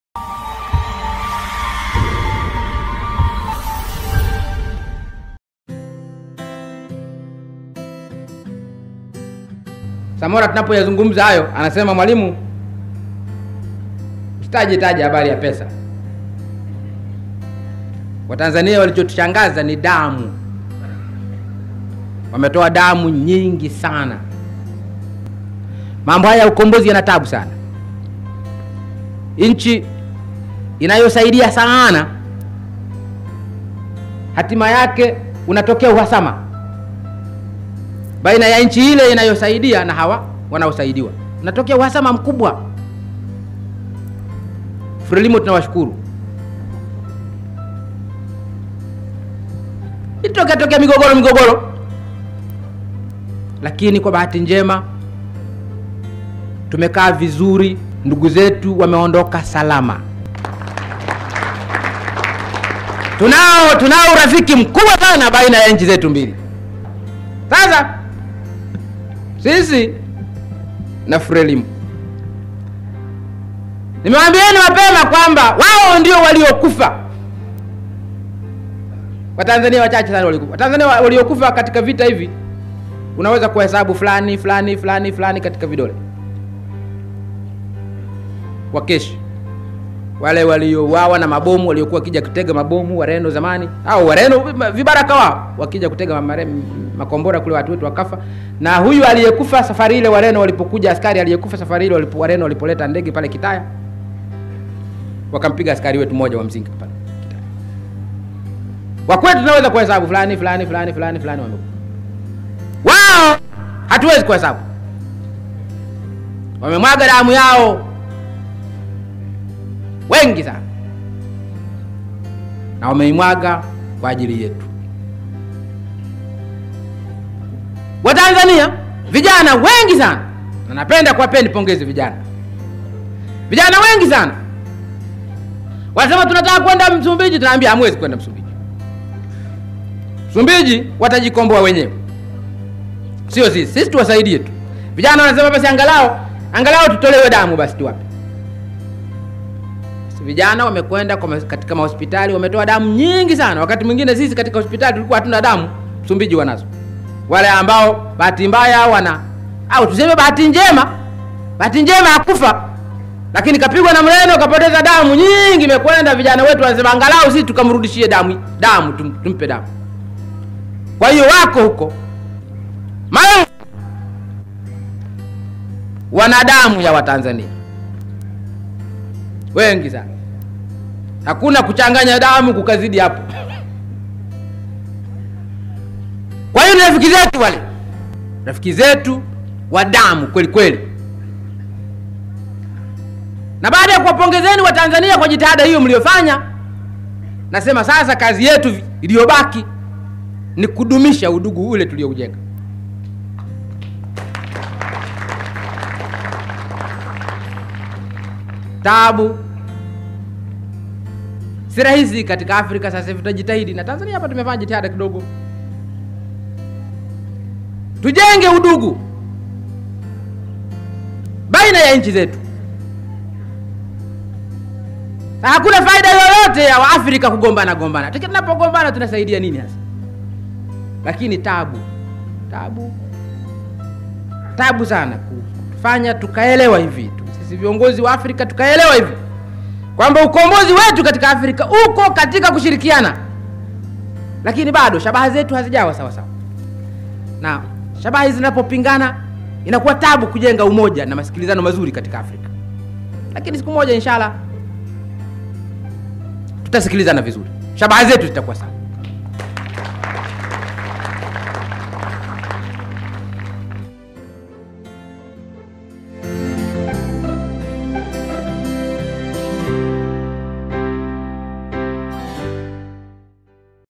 "Msumbiji Wana Damu ya Watanzania" Mwl. Nyerere
Mchango wa Tanzania katika ukombozi wa nchin za kusini mwa Afrika ikiwemo Msumbiji ni mkubwa zaidi ya vile unayoweza kufikiri! Mwalimu anaeleza namna Watanzania walivyokua wanawiwa kuisaidia Msumbiji sio kwa Mali peke yake bali hata kwa Hali kwa kuchangia Damu kwa wapigania Uhuru wa Msumbiji chini ya Chama cha FRELIMO, Kumbukumbu hizi sio tu zinatupa uelewa kuhusu harakati za Ukombozi bali zinatujenga kuendelea kudumisha udugu na umoja katika Afrika.